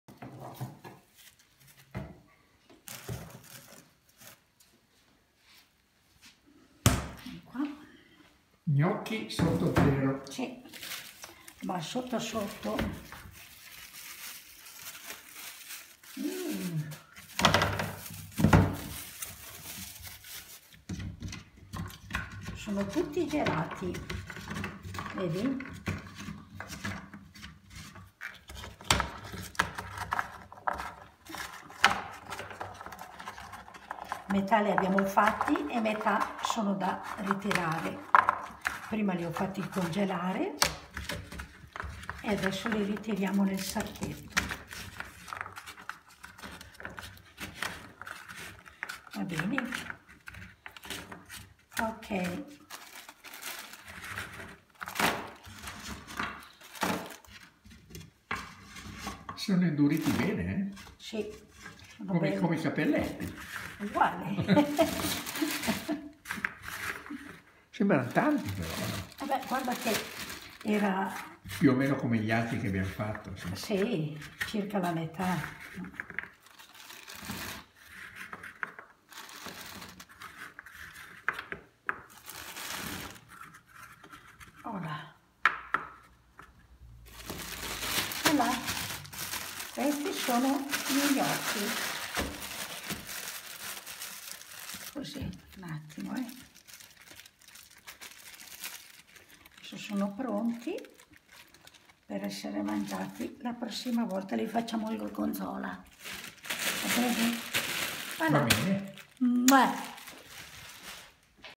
Congelare gli Gnocchi di Patate
Come congelare e fare scorta di Gnocchi Freschi!
Quando  facciamo gli gnocchi,  se vogliamo farne un po' di piú , per poi averla giá pronta quando non abbiamo tempo, possiamo congelarla ...
La soluzione migliore é congelarla nei vassoi e poi quando é bella dura, staccarli ed inserirli nel sacchetto, cosí non prendono troppo spazio!!

A presto,

Buon Appetito!

Ciao

Ecco gli utensili utilizzati: